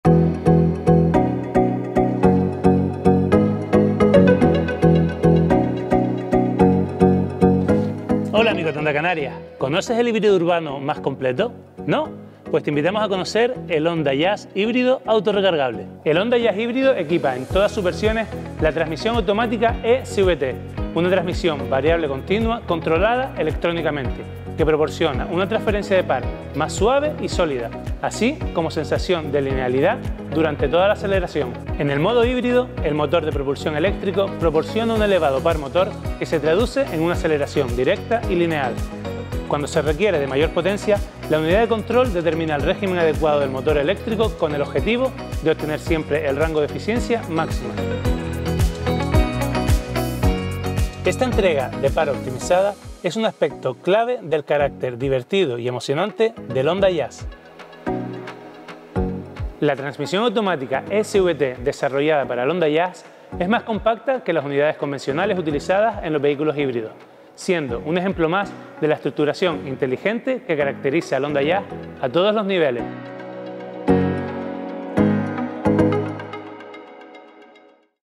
Honda Jazz Híbrido
¡Descubre el Honda Jazz Híbrido!

Nuestro Dealer Network & Sales Manager, Francisco Ramírez, nos habla del híbrido urbano más completo.

El Honda Jazz cuenta con la avanzada tecnología híbrida e:HEV de Honda. Es ágil, suave y eficiente. No te pierdas cada detalle.

¡Esperamos tus preguntas sobre nuestro modelo!